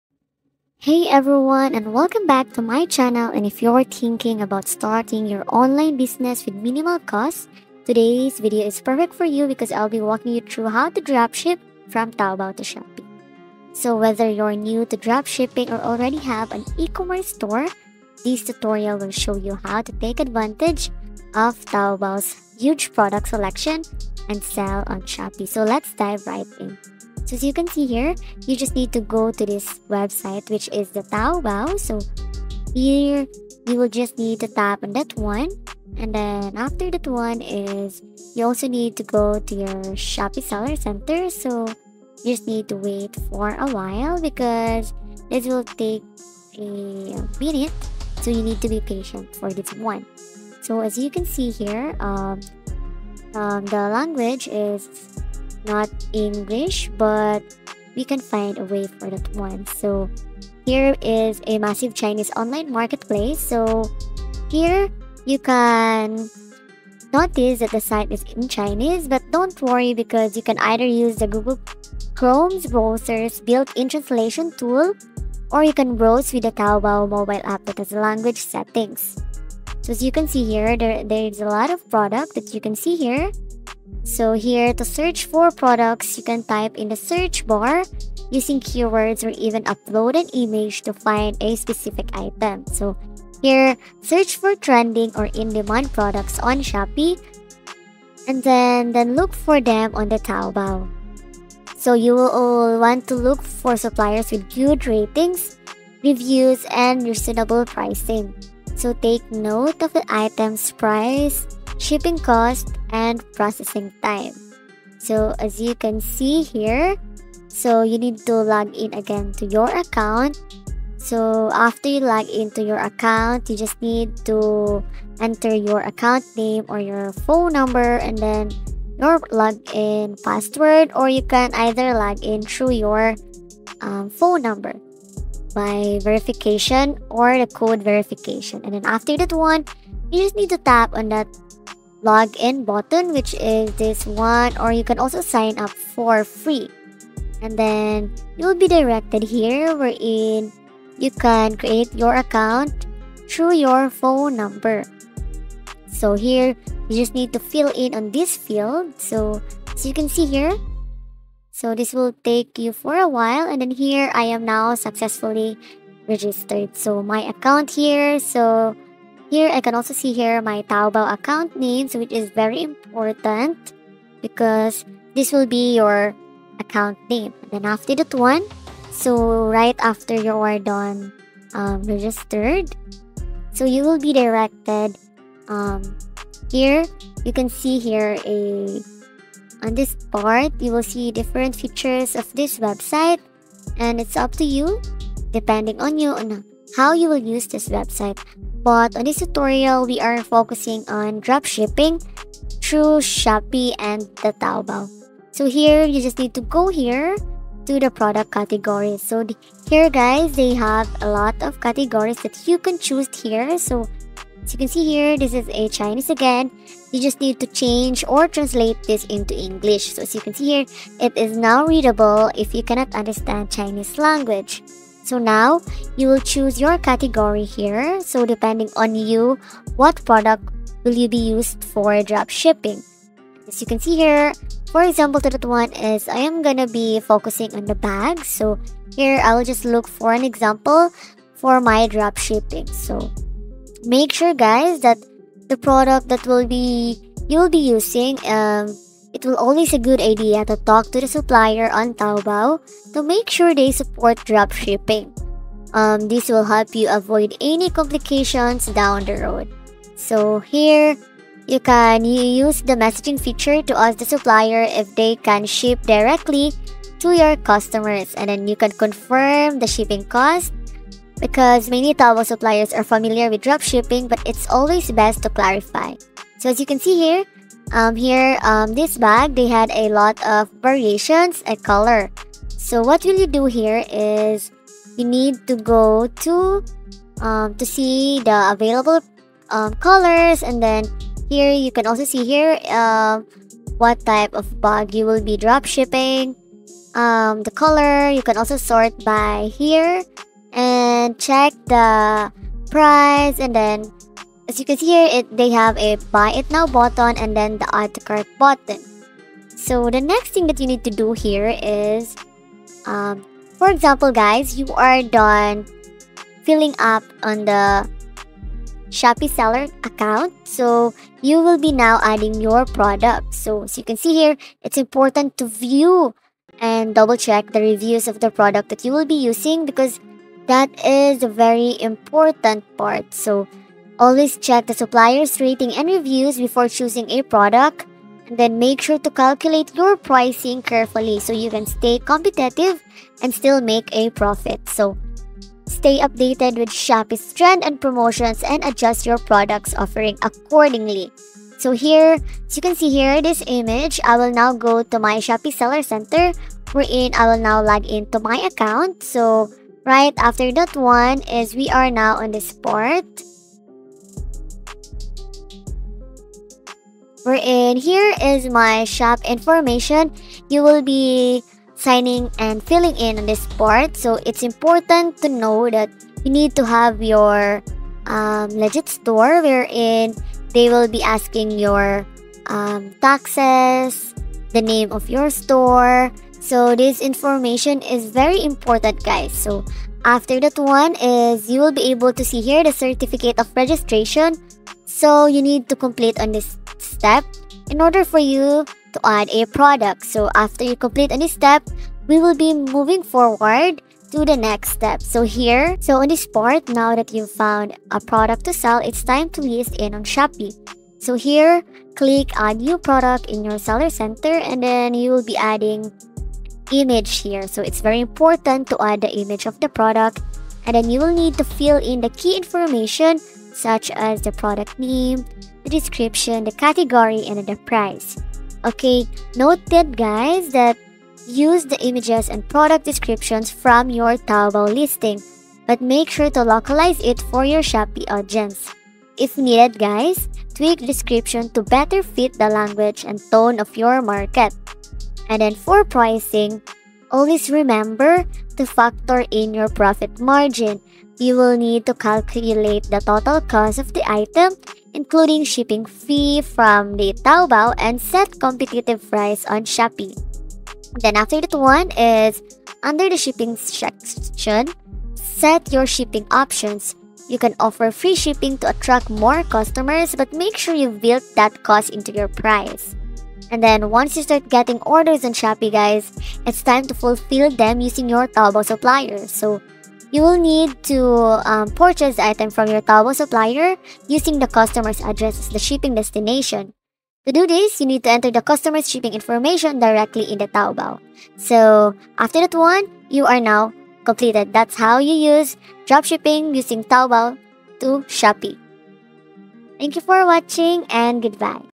How to Dropship from Taobao to Shopee (Full Guide)
Learn how to dropship from Taobao to Shopee with this full guide. Discover the step-by-step process to successfully dropship products and grow your online business!